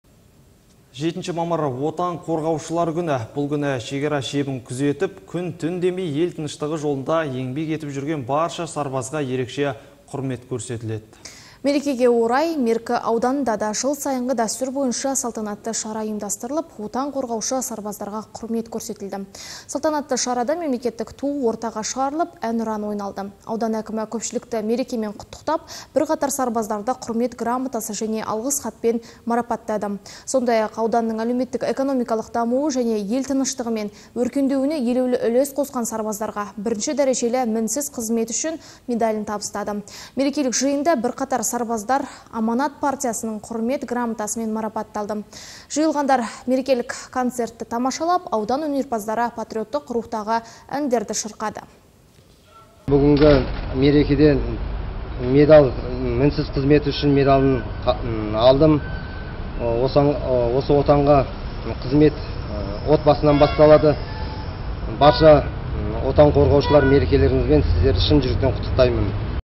Отан қорғаушыларға құрмет көрсетілді
7-мамыр – Отан қорғаушылар күні. Бұл күні шекара шебін күзетіп, күн-түн демей ел тыныштығы жолында еңбек етіп жүрген барша сарбазға ерекше құрмет көрсетіледі.

Мерекеге орай, Меркі ауданында да жыл сайынғы дәстүр бойынша салтанатты шара ұйымдастырылып, Отан қорғаушы сарбаздарға құрмет көрсетілді.